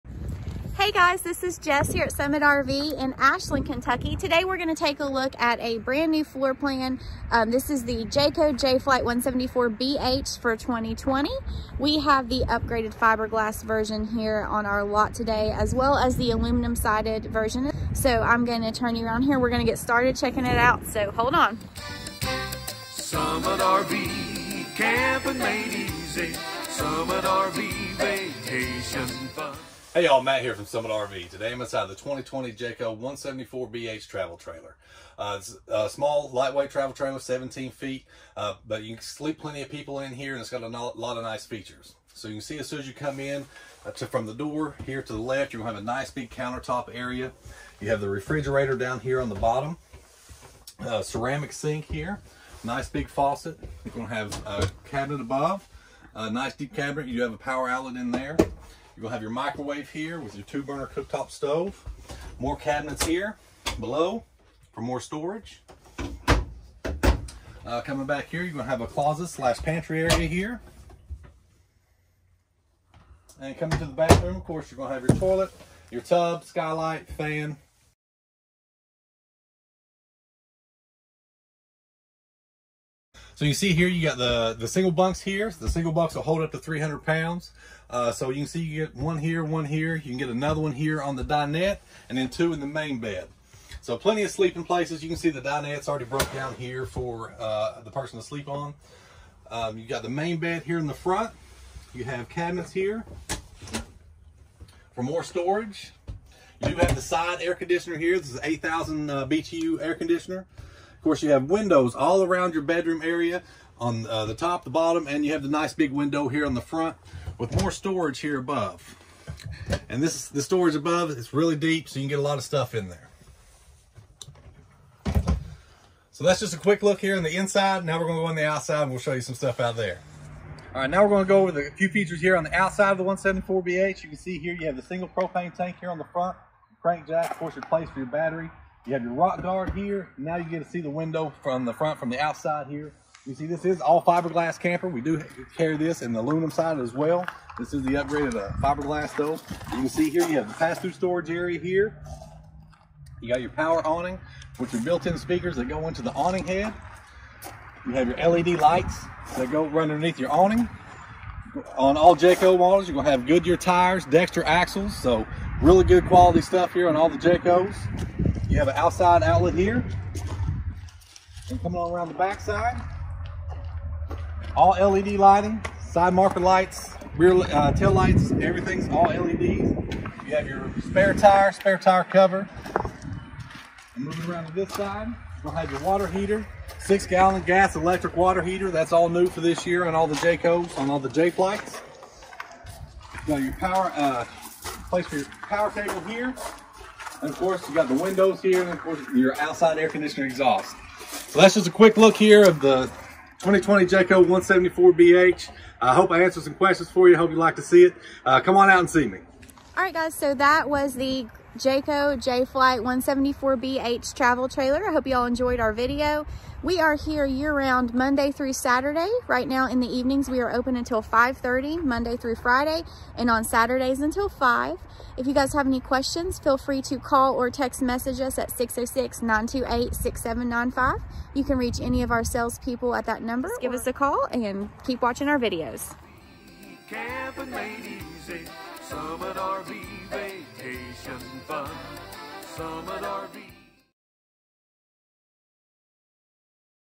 New 2020 Jayco Jay Flight SLX 174BH Travel Trailer at Summit RV in Ashland, KY
2020 174BH Features and Options
Jayco Jay Flight SLX 7 travel trailer 174BH highlights:

Queen Bed
Microwave Oven
Kitchen Pantry
Large Front Window
LED Lights

A great choice for your family of five is this Jay Flight SLX 7 travel trailer! The bunk beds and queen-size bed provide a comfortable spot for you to sleep at night, and a fifth camper can be comfortable on the booth dinette once it has been folded down into a bed. Grabbing a bite to eat is simple with the two-burner range and microwave oven. Pop a bag of popcorn in the microwave for a late-night snack and enjoy a card game or two inside at the table. The 10' electric awning with LED lighting will provide a great outdoor space both day and night.

With any Jay Flight SLX 7 travel trailer, your vacationing couldn't be simpler. These units provide you with all of the comforts needed along with complete reliability on the road. You will appreciate the seamless Magnum Truss roof system, the American-made Goodyear tires, and the Keyed-Alike entry and baggage doors so that you only need to keep track of one key! On the inside, you will love the solid hardwood cabinet doors, LED lighting throughout, and the residential-height bed platforms! And, for your temperature-control comfort, there is a wall-mounted 8,000 BTU AC unit so that you can stay cool even on the hottest summer day!

Packages:

Customer Value Package

-Goodyear Tires

-6Gal G/E DSI Water Heater

-2 Burner Cooktop

-3 Cu. Ft. Refrigerator with Temperature Control

-Bath Skylight

-Front Diamond Plate

-Keyed Alike Entry & Baggage Doors

-Microwave

-Pleated Shades

-Power Awning with LED Lights and Speakers

-Range Hood

-Spare Tire with Cover

-Stabilizer Jacks (2)

-Water Heater Bypass

Additional Included Options:

-Bedspread

-Fiberglass Sidewalls